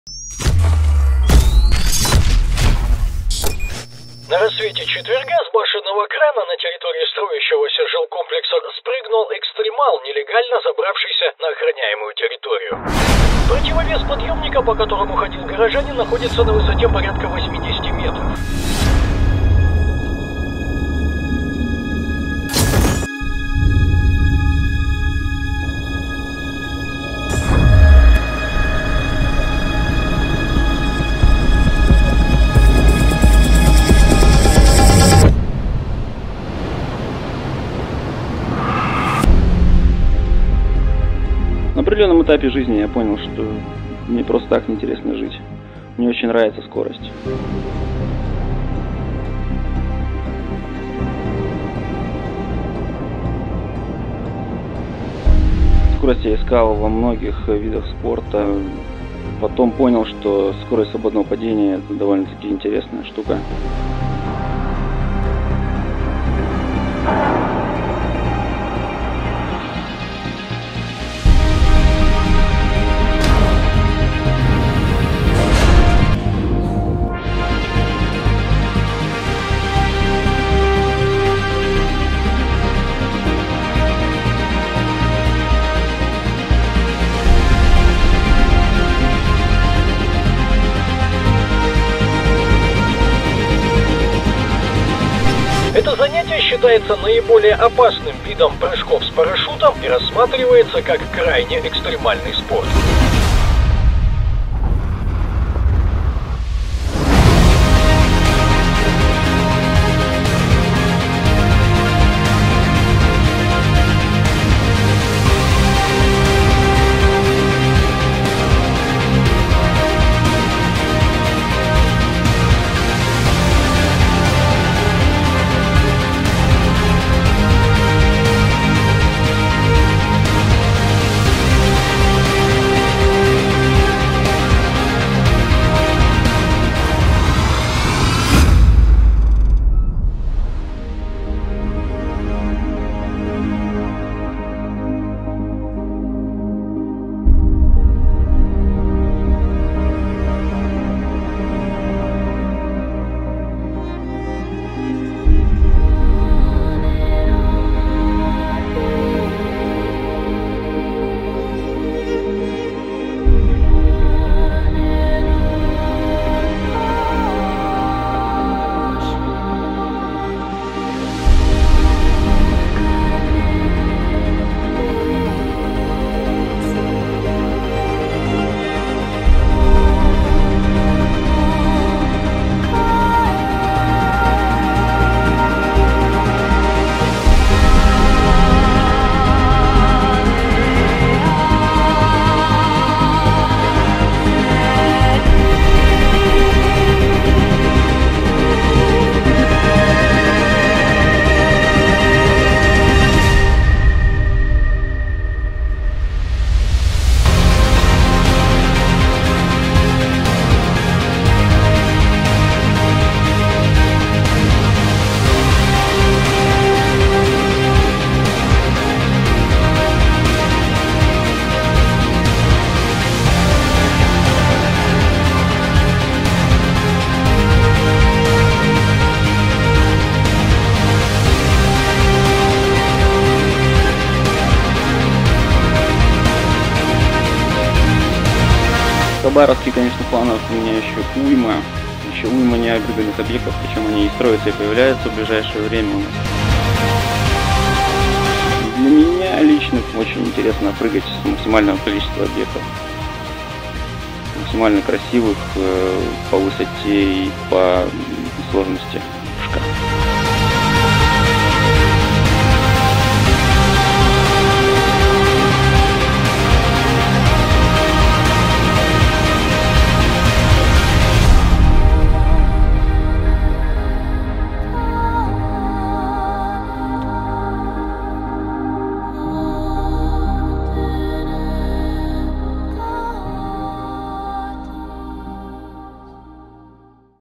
Up/Down || BASE Jump
Все объекты на видео находятся в г. Хабаровск, Россия

Активно начали работать над созданием видео с середины лета. Проект был достаточно интересным, поэтому сил в него вкладывалось очень много. Изначально съемки шли как обычно – сплошная импровизация (снимаю что вижу). Затем был составлен сценарий, чтобы видео приобрело смысл. За несколько месяцев съемок накопилось такое количество материала, из которого, если добавить к нему еще и все отснятые по сценарию сюжеты, получался короткометражный фильм, продолжительностью минут на 25.
В итоге многие сцены было решено не включать в видео, тем самым продолжительность сократилась до 5 минут – что уже не так утомляет зрителя при просмотре.
Из всего материала в эти 5 минут я попытался вместить все самое основное и максимально приблизить зрителя к тому, что видит спортсмен. Простыми словами - помочь взглянуть на это “сумасшествие” его глазами.
Работа над данным проектом – была отличной возможностью познакомиться с людьми, которые живут интересной жизнью, которые также как и я стараются взобраться куда-нибудь повыше, чтобы увидеть наш город с самых необычных ракурсов, взглянуть на него по иному, да и просто насладиться высотой.
Все что нам удалось запечатлеть за несколько месяцев работы, возможно, вскоре мы покажем в короткометражном фильме, который задумывался изначально.
А пока мы хотим поделиться нашим проектом Up/Down. Надеюсь вам понравится.
Thursday at sunrise, an extreme sports enthusiast, illegally jumped off a crane located on a security guarded construction site of an apartment complex.The counterweight of the crane, on which the person was walking on, was approximately 80 meters high. "At a certain point of my life, I came to the realization that I absolutely love speed. I have tried to fulfill my desire for speed in many types of sports. Eventually, I encountered the speed of free falling. Free falling is considered to be one of the most dangerous sports, even more so than sky diving. In Khabarovsk, there are still plenty of sites that I am planning to jump off in the near future. It is a goal of mine to jump off as many beautiful heights as I can."